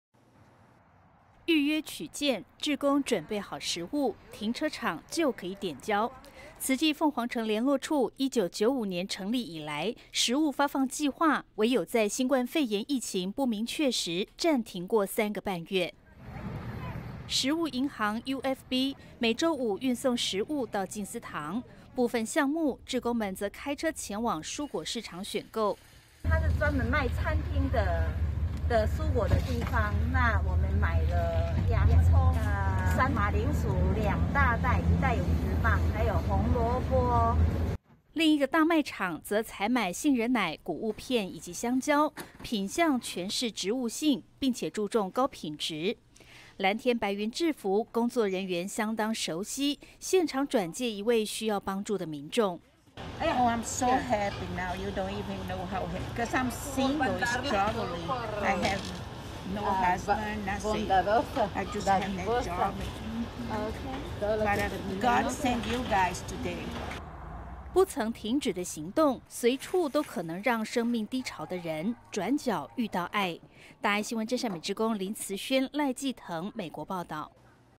州政府挹注資金 鳳凰城食物發放添動力
美國慈濟鳳凰城聯絡處，自1995年成立以來，每周都有三天時間，讓需要幫助的家庭前來領取食物。食物的來源，包括食物銀行、善心人士捐贈、以及志工到大賣場採購補充。 今年，亞利桑那州政府額外提供一筆資金給食物銀行合作的夥伴，慈濟也申請到了，不中斷食物發放計畫。

預約取件，志工準備好食物，停車場就可以點交。

慈濟鳳凰城聯絡處1995年成立以來，食物發放計畫，唯有在新冠肺炎疫情不明確時，暫停過三個半月。

食物銀行UFB 每周五運送食物到靜思堂，部分項目，志工們則開車前往蔬果市場選購。

慈濟志工 陳慈盛：「它是專門賣餐廳的蔬果的地方，我們買了洋蔥 馬鈴薯2大袋，一袋有10磅 還有紅蘿蔔。」

另一個大賣場則採買杏仁奶、穀物片、以及香蕉，品項全是植物性，並且注重高品質。藍天白雲制服，工作人員相當熟悉，現場轉介一位需要幫助的民眾。

時薪人員 安娜：「我太高興了 你可能不知道，我單身的處境，我沒有先生 什麼都沒有，我只能這樣打工，感謝主 我今天遇到你們。」

不曾停止的行動，隨處都可能讓生命低潮的人，轉角遇到愛。

真善美志工 林慈宣 賴濟騰 美國報導